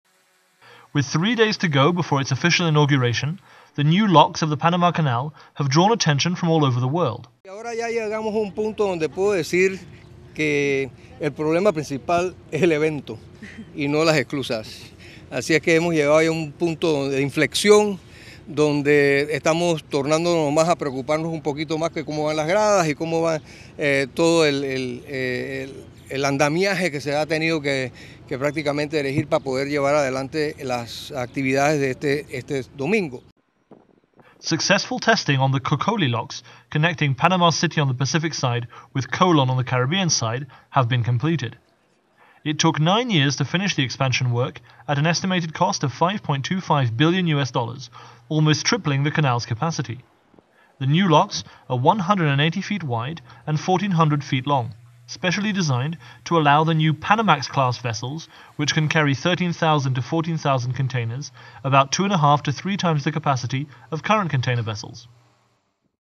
Panama counts down inauguration of new locks of Panama Canal
With 3 days to go before its official inauguration, the new locks of the Panama Canal have drawn the attention from all over the world.

(Soundbite) Jorge Luis Quijano, Panama Canal Administrator
"Now we come to a point where I can say the main problem is the event and not the locks. So we have reached a turning point, where we are becoming more worried about whether everything will go smoothly on Sunday (when the locks are officially inaugurated)."

Successful tests were conducted for the Cocoli Locks connecting the Panama City on the Pacific side with Colon on the Caribbean side.

It took 9 years to finish the 5.25 billion U.S. dollars  expansion work, almost tripling its capacity.
The new locks are 180 feet wide and 1,400 feet long, specially designed for New Panamax-class vessel, which can carry 13,000 to 14,000 containers, about 2.5 to 3 times those of current ones.